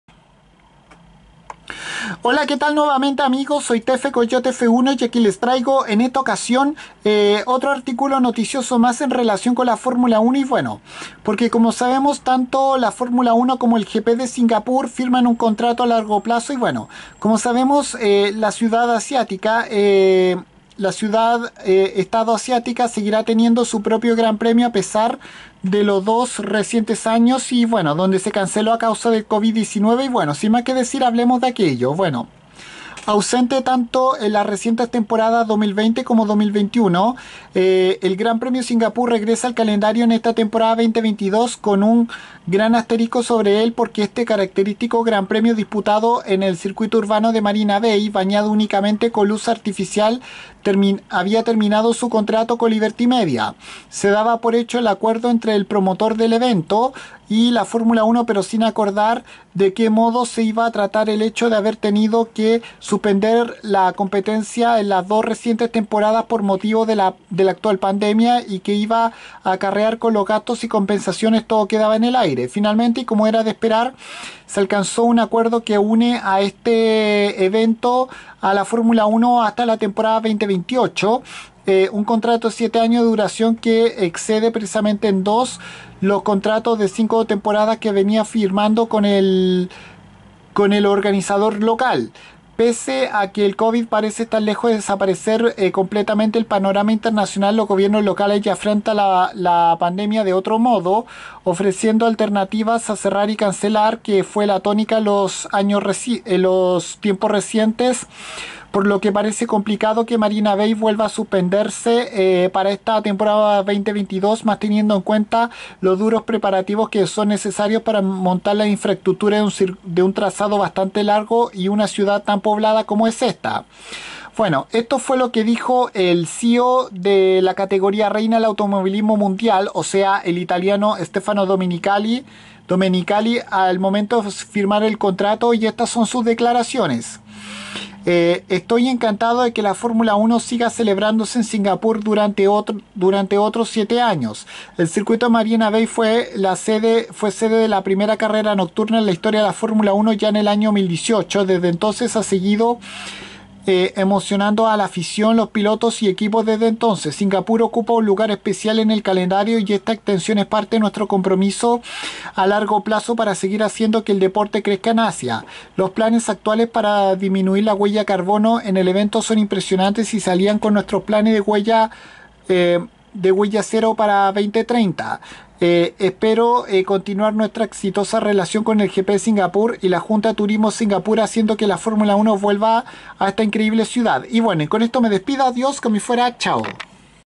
La F1 y el GP de Singapur firman un nuevo acuerdo hasta 2028
La ciudad estado asiática estará dentro del calendario de la Fórmula 1 hasta la Temporada 2028 y esta nueva Temporada 2022 regresará luego de 2 años de cancelación por la actual pandemia de Covid19